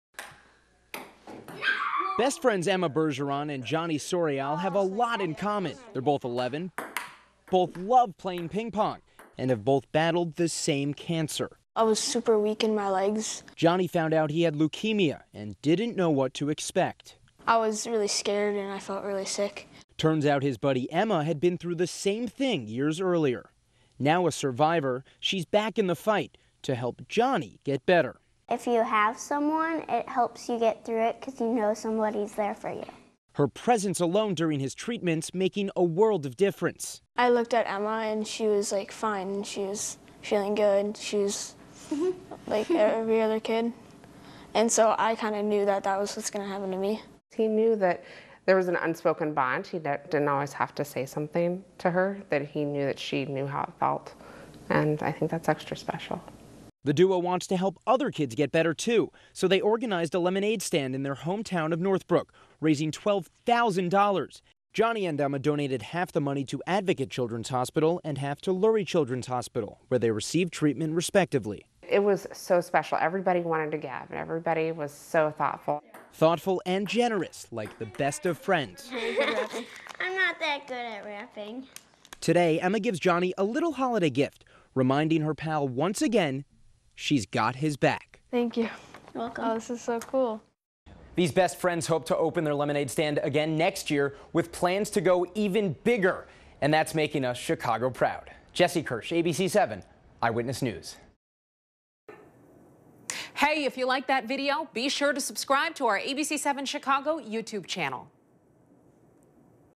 Northbrook kids $12k for cancer patients
Best friends Emma Bergeron and Johnny sorial have a lot in common: They're both 11 years old, both love playing ping-pong, and have both battled Leukemia. Jesse Kirsch reports on the difference they're making for other kids fighting cancer